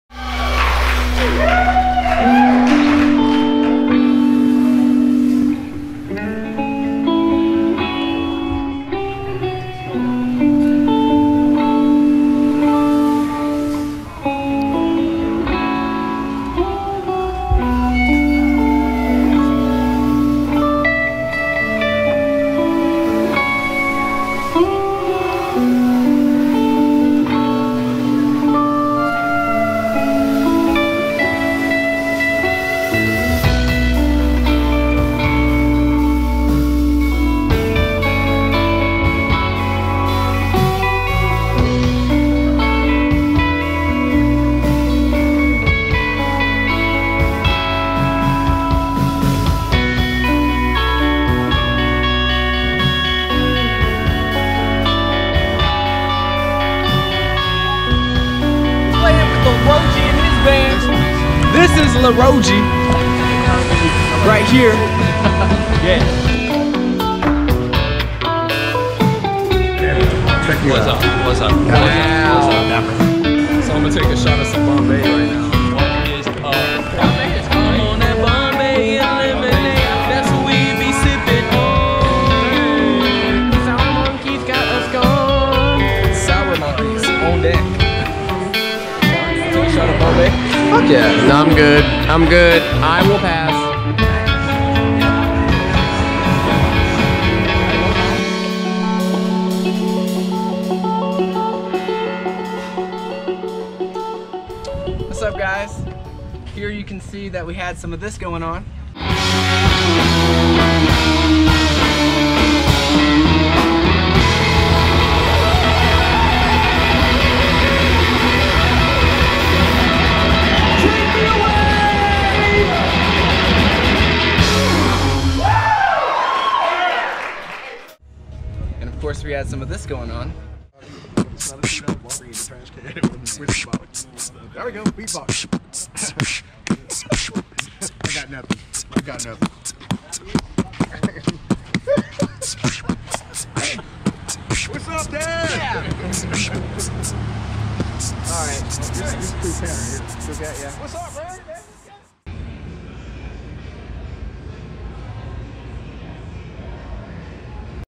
Mixed Tribe @ The Alamo!
We played at The Alamo for the first time recently, and here are some clips of us and how it went! Enjoy.

Links!!